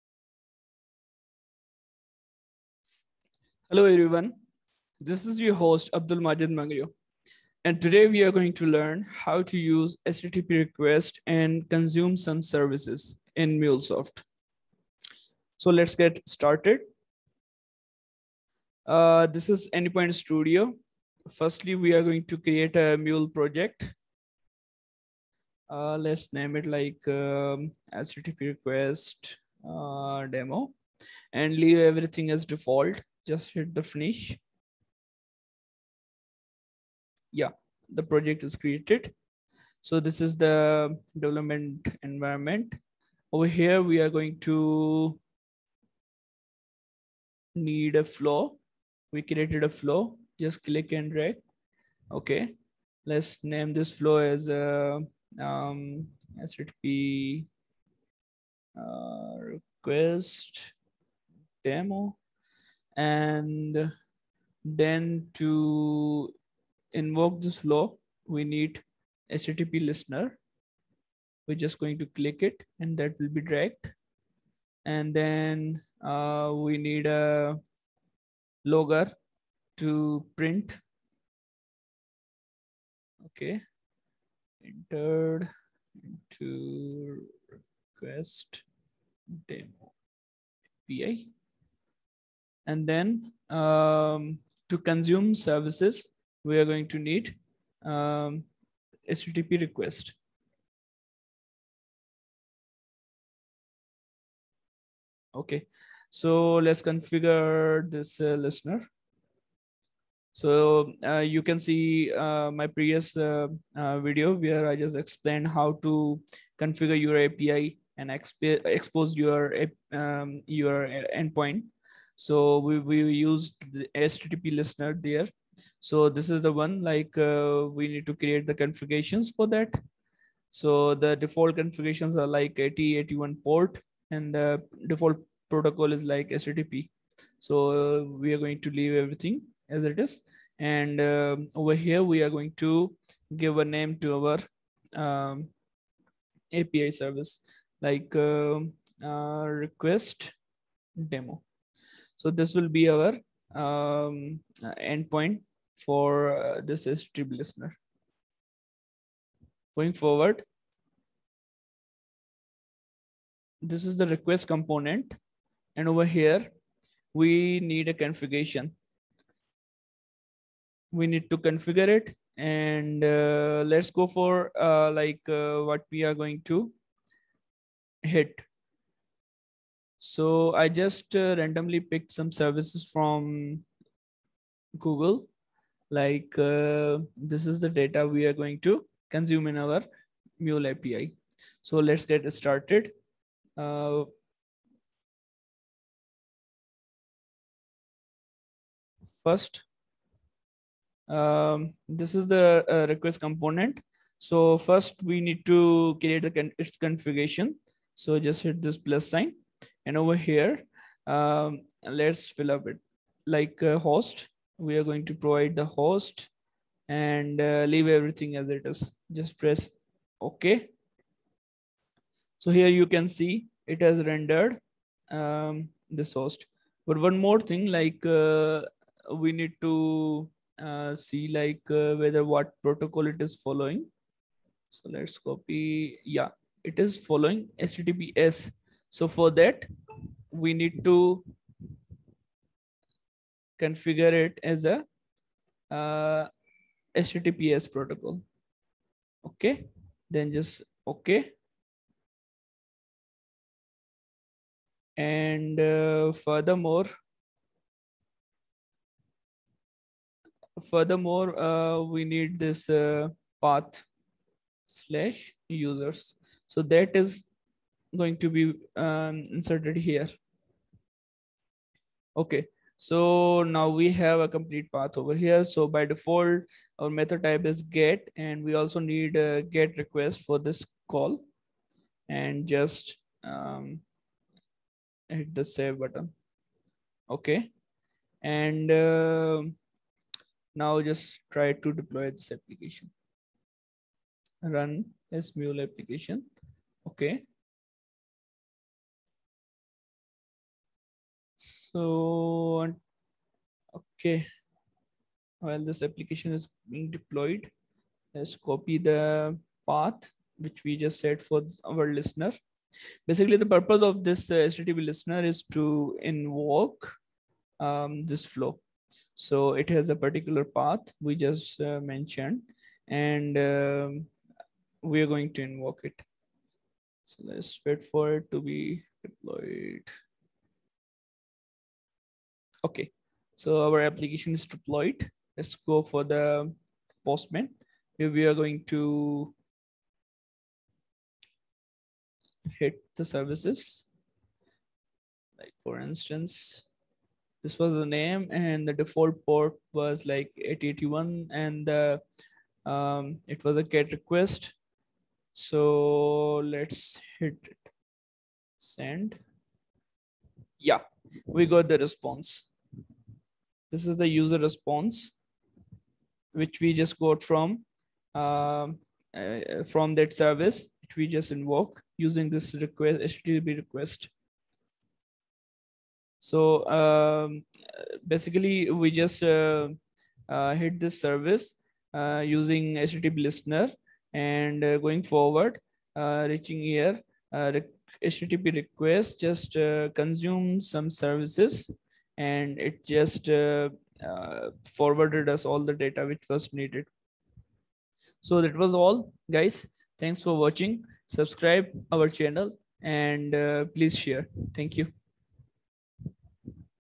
How to Consume Services using HttpRequest in Mulesoft.
This is a very interesting topic, to learn Mulesoft and try to Consume Services using HttpRequest in Mulesoft